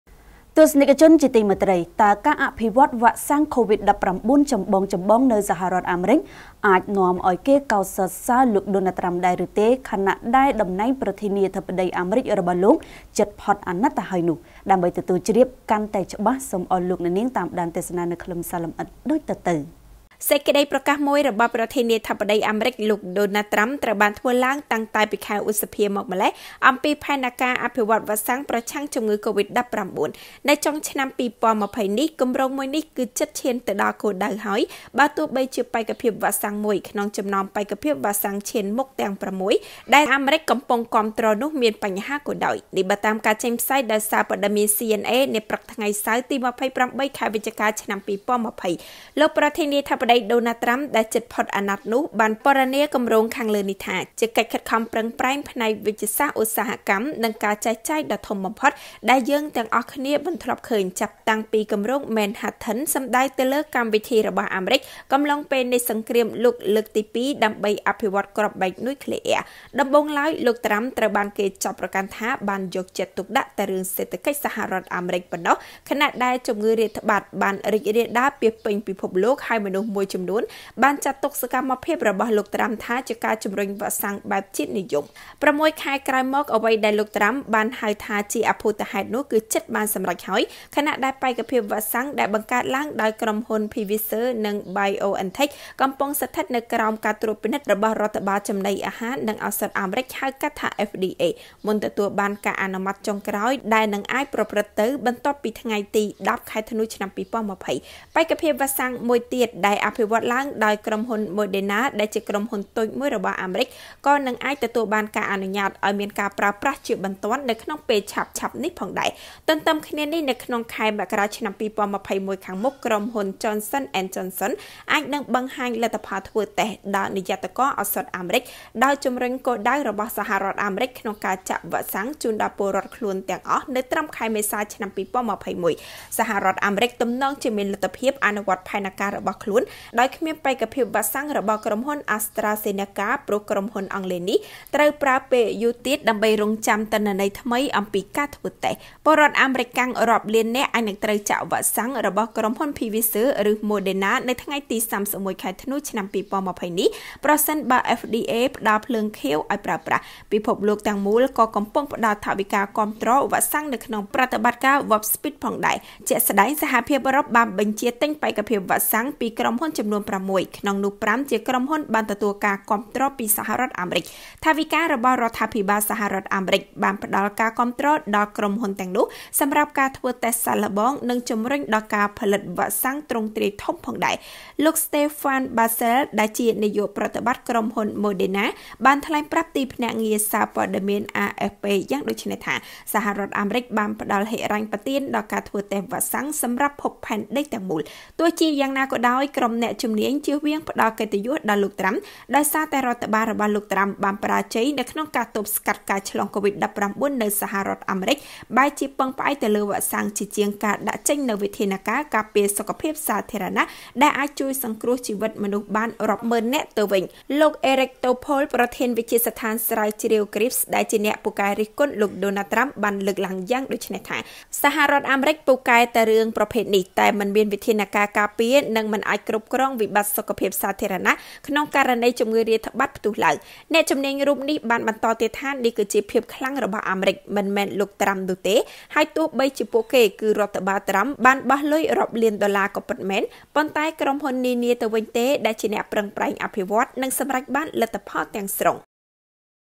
ស្វែងយល់! តើការអភិវឌ្ឍវ៉ាក់សាំងកូវីដ-១៩ ចម្បងៗនៅអាមេរិក អាចនាំឲ្យគេកោតសរសើរលោក ដូណាល់ ត្រាំ ដែរឬទេ?
* រាយការណ៍ព័ត៌មានដោយកញ្ញា យឹម ចាន់ស្រីវីន  និងអ្នកនាង យ៉ង់ សក្គុណា , ទូរទស្សន៍ព័ត៌មាន Fresh News